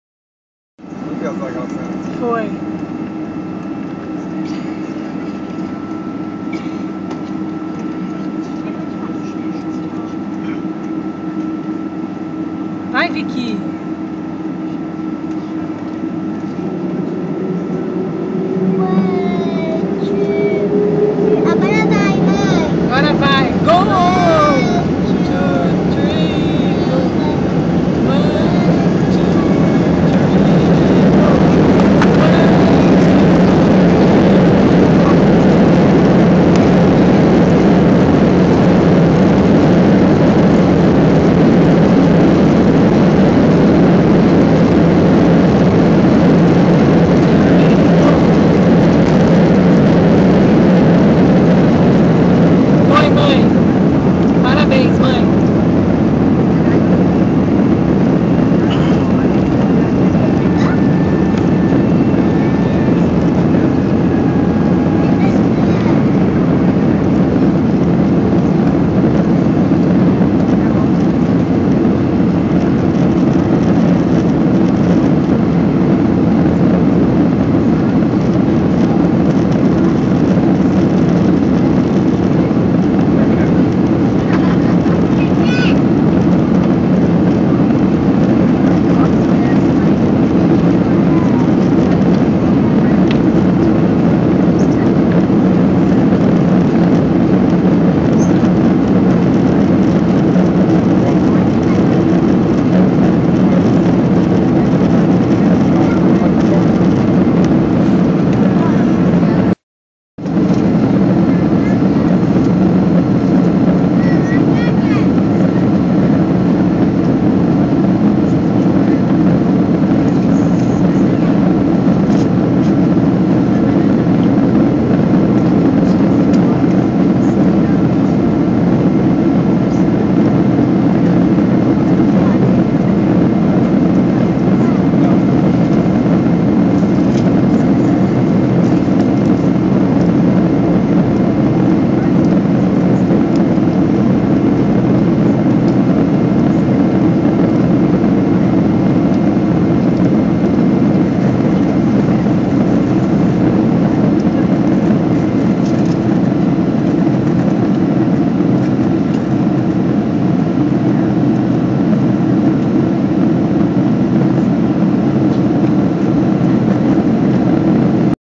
RC - EP00002 - Powerful SHORT takeoff on American 767-200 from MCO to MIA
Early morning takeoff from Orlando (KMCO) to Miami (KMIA). Power and speed for a very short takeoff run on a fully loaded 762 39 minute hop.